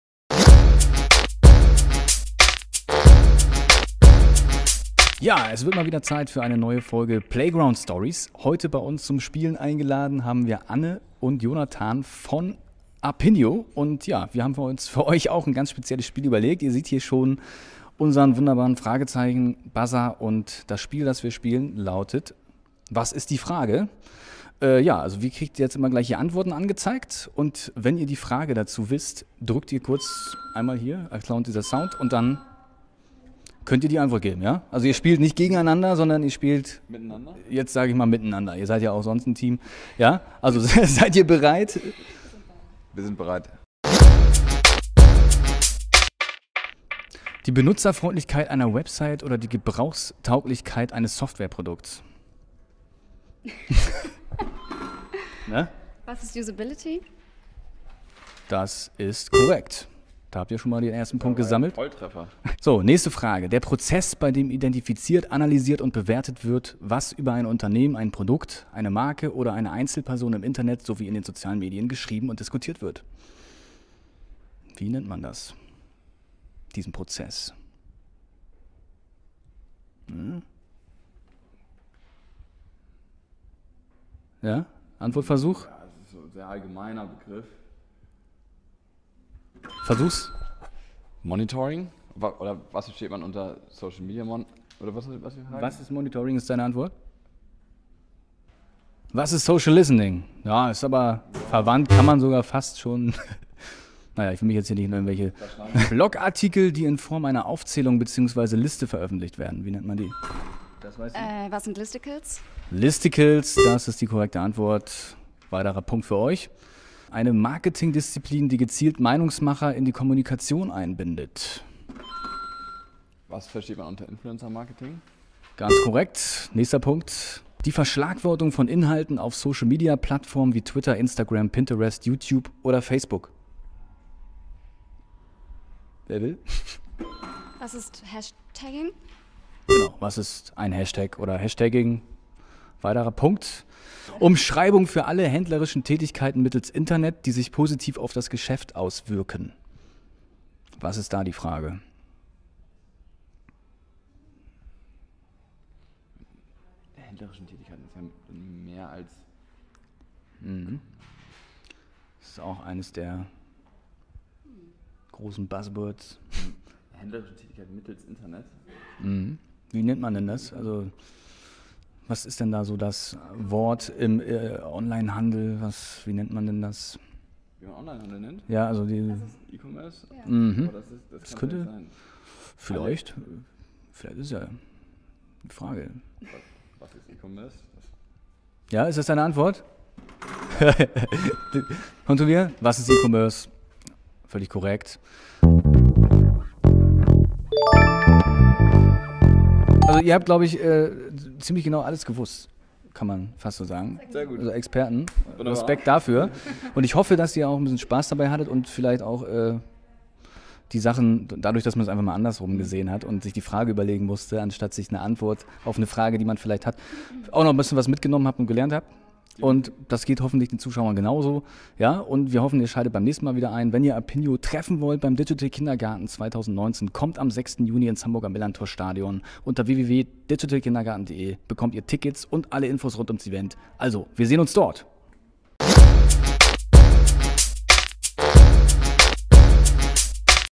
Playground Stories #03 – Was ist die Frage? feat. Appinio
Erinnert ihr euch noch an die TV-Show Jeopardy? Wir haben den alten Gameshow-Klassiker gemeinsam mit den Mobile-Research-Experten Jonathan Kurfess und Anne-Kathrin Richter von Appinio wieder aufleben lassen und eine DK2019-Version gespielt.